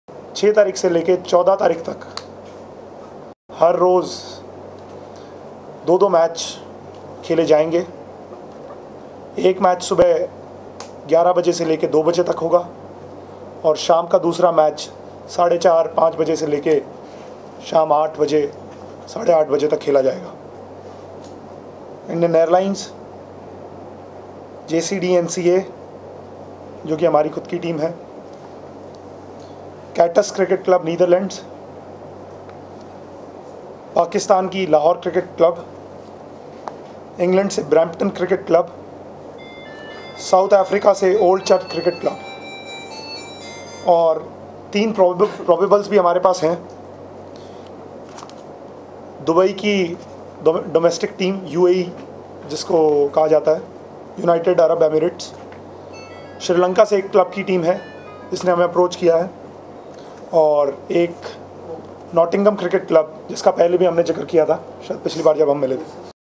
Digvijay Chautala announces International cricket at JCDV
Digvijay announces International cricekt matches at JCDV from Sept. 6th 2008